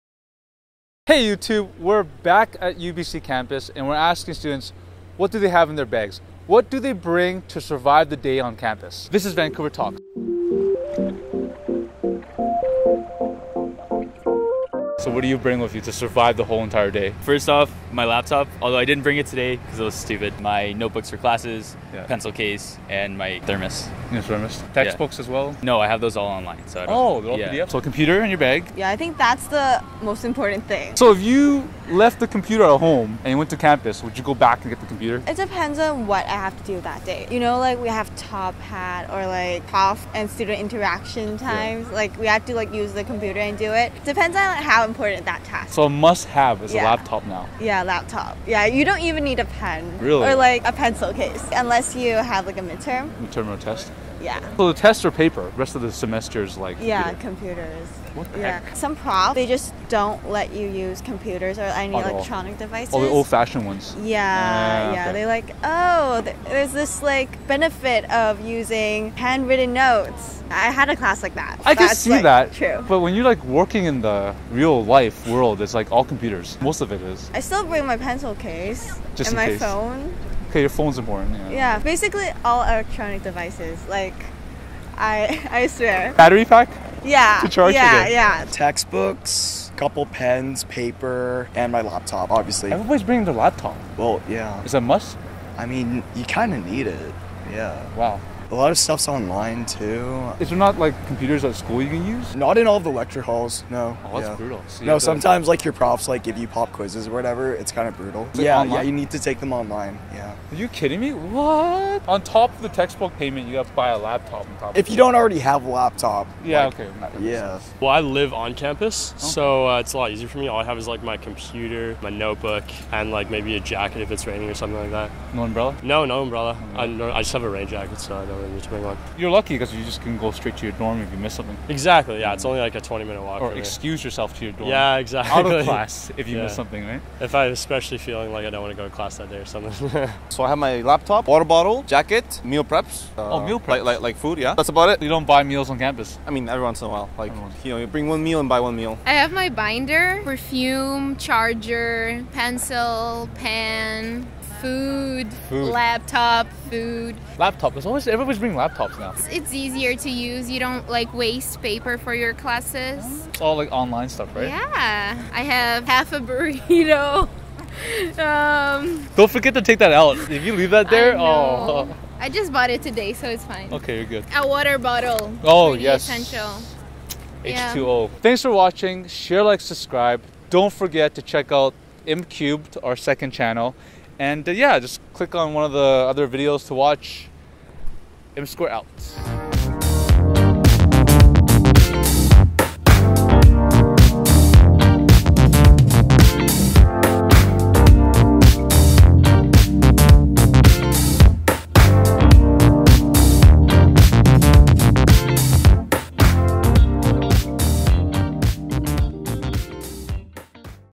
What's in Your Bag [UBC Campus Edition] - Vancouver Talks
We ask students What's in your bag? What do you carry with you to survive school and being on campus during the day?

[Shoutouts]
Hosts: Joe
Camera: Joe/@alexandergmatthews
Edit: Joe